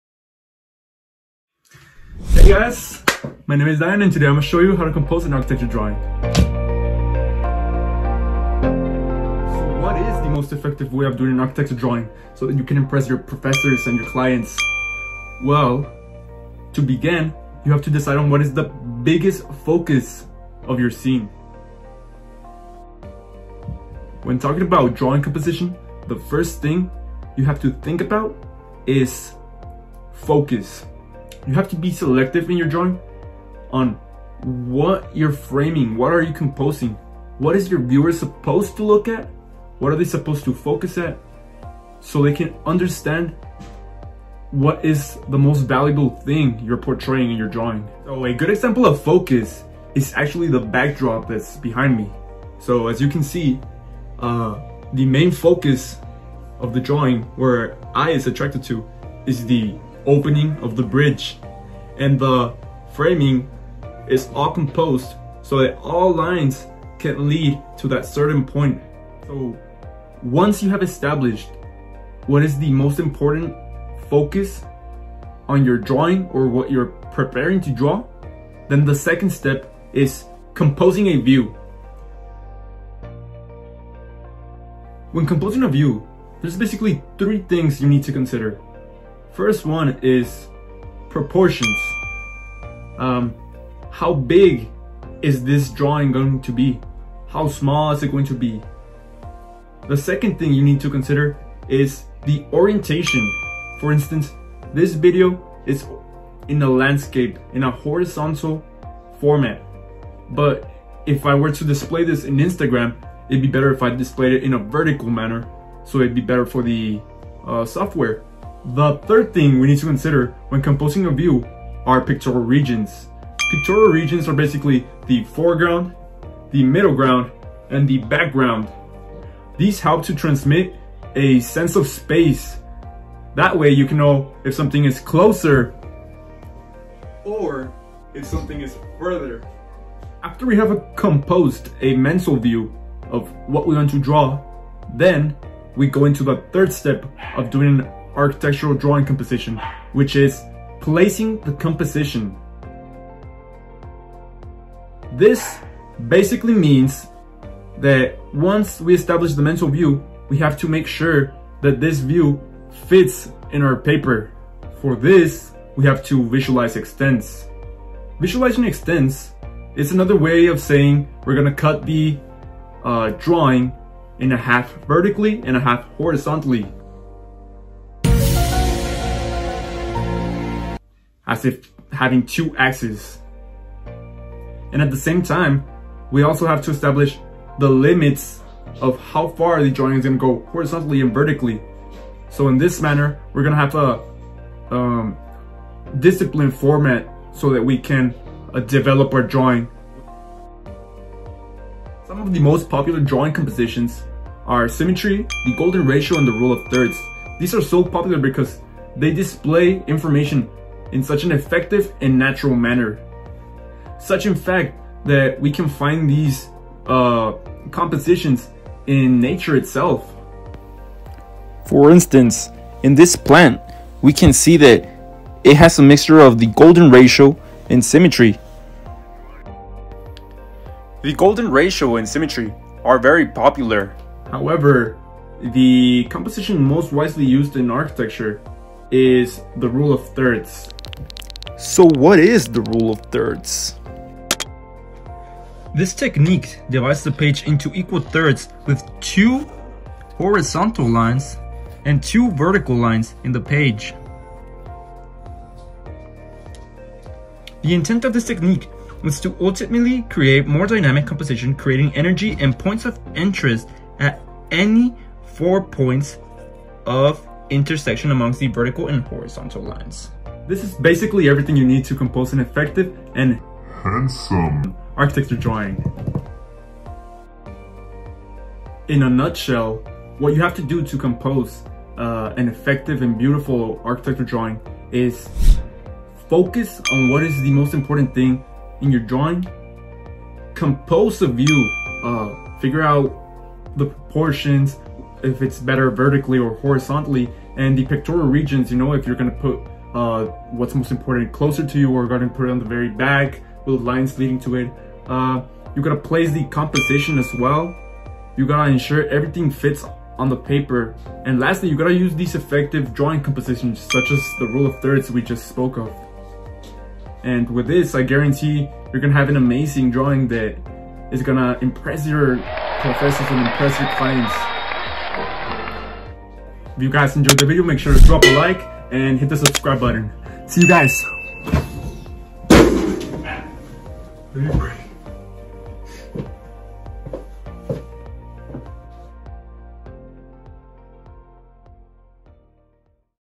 How to IMPROVE your drawing composition FAST
How to tutorial on how to achieve the best architectural compositions based on Chapter 10 of Architectural Graphics 6th Edition by Francis D.K. Ching. Sit, relax, enjoy, and learn!